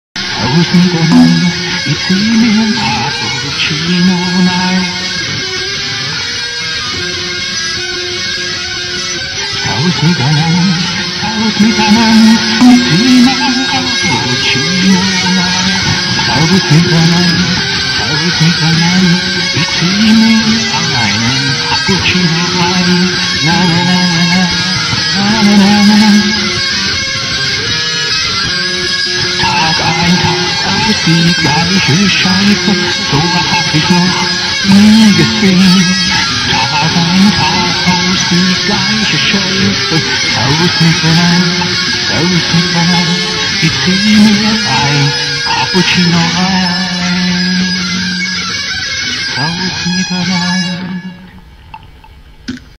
Teil 3 Wochenendmusik(WochenendPunk)
Habe Gitarre Geige gespielt und gesungen
viel Spaß ;-)